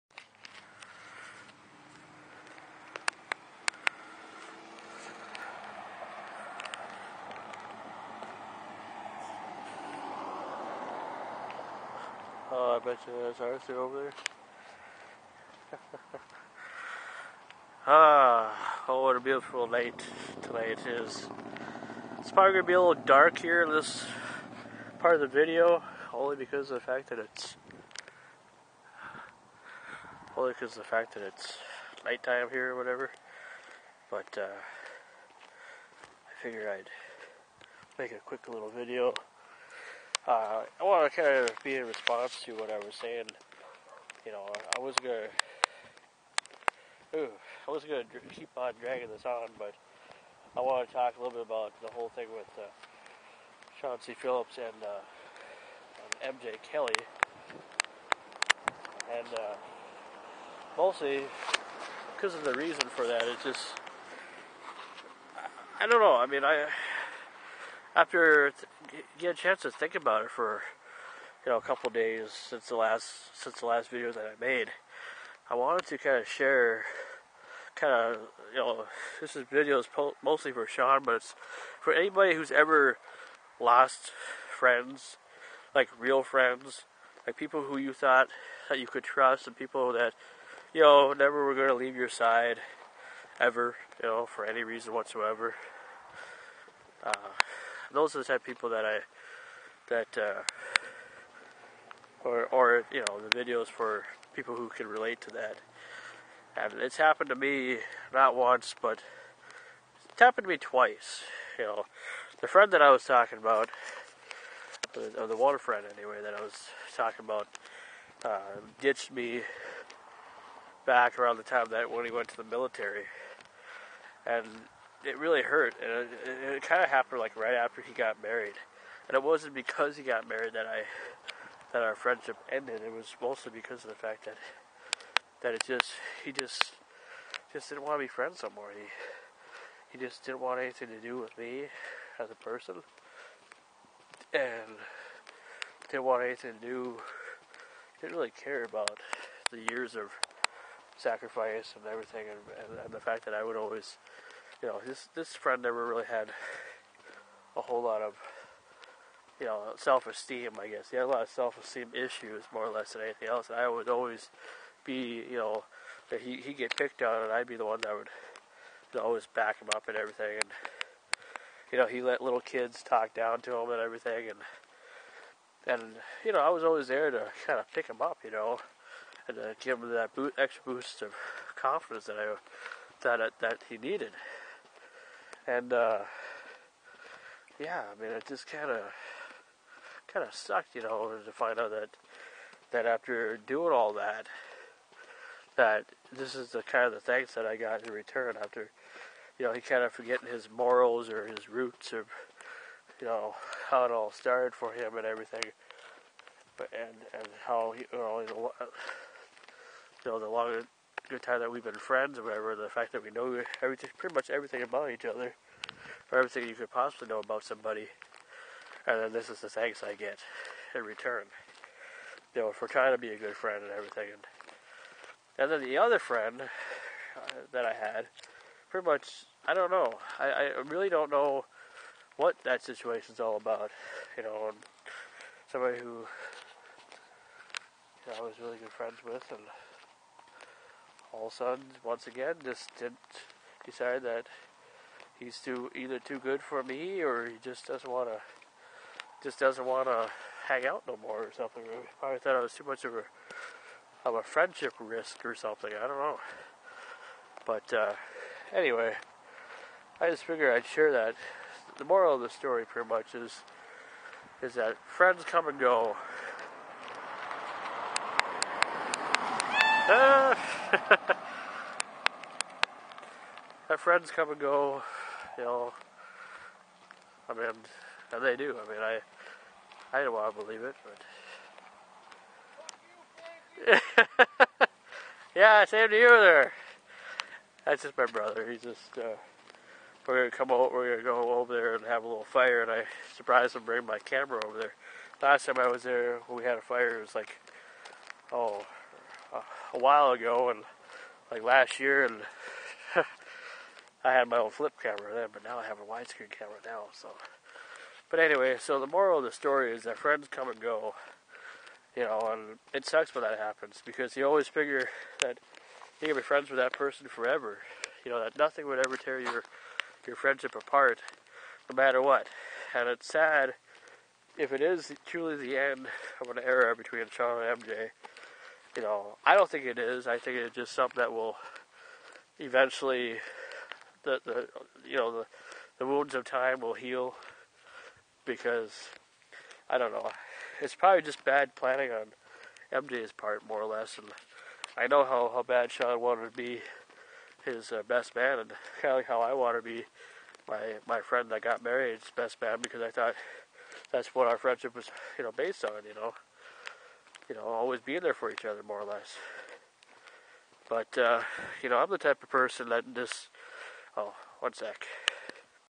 CAMPFIRE CONVERSATIONS 2! (HD) Part 1
In this video I head over to My Bro's House to have our 1st Campfire of the year. Also a special Message to Shawn C. Phillips.